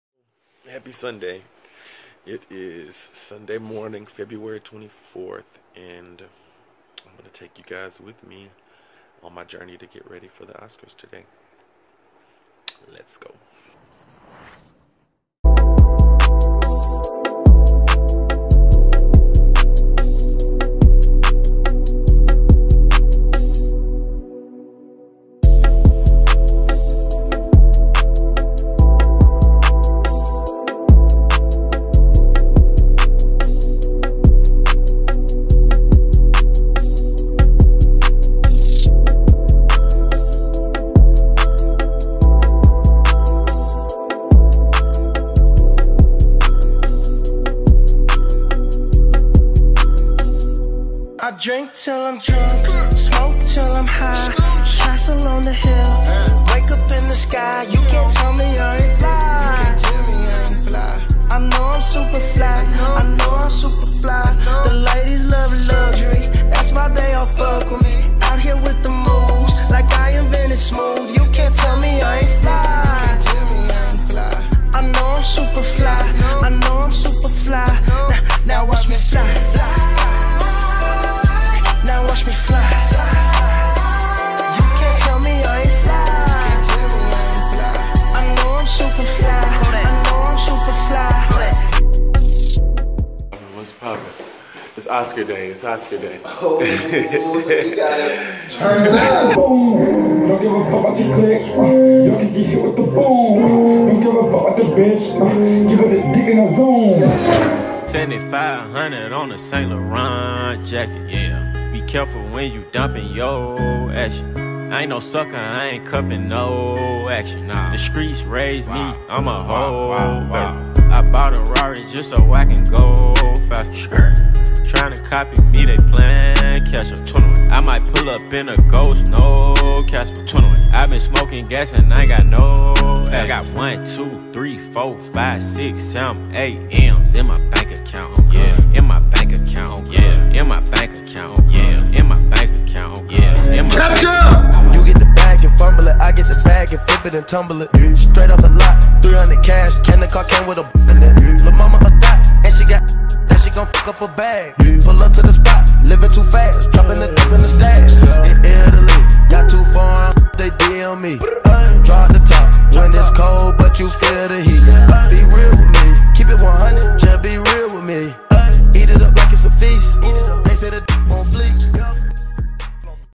Roger Sterling - Oscar Prep 2019 - Securing the Bag
Rare look behind the scenes in the prep for the 2019 Oscars.
Thank You @parriswagner for always keeping me on point @Fswaver for the Fashions. Mike thanks for printing my tickets. Enjoy!!